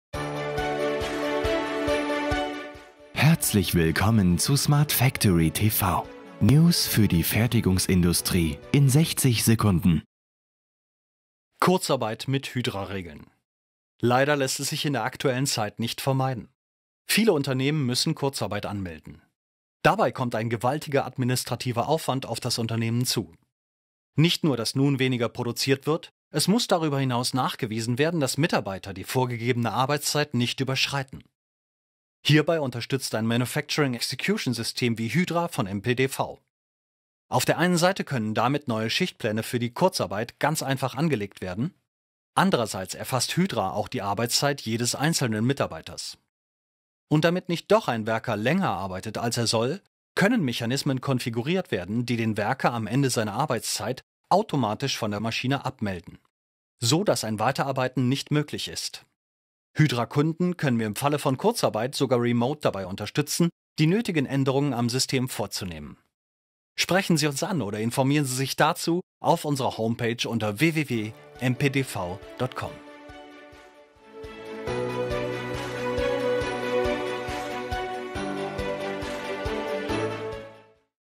Smart Factory TV - Kurzarbeit mit HYDRA regeln
Wie kann man Arbeitszeiten in der Fertigung ganz einfach aufzeichnen? Wie kann gewährleistet werden, dass in Zeiten vorn Kurzarbeit, die vorgegebene Arbeitszeit nicht überschritten wird?

Mit HYDRA, dem Manufacturing Execution System von MPDV, können neue Schichtpläne ganz einfach angelegt werden und die Arbeitszeiten jedes Mitarbeiters erfasst werden. Auch ein automatisches Abmelden von Maschinen ist mit HYDRA möglich.

We create smart factories.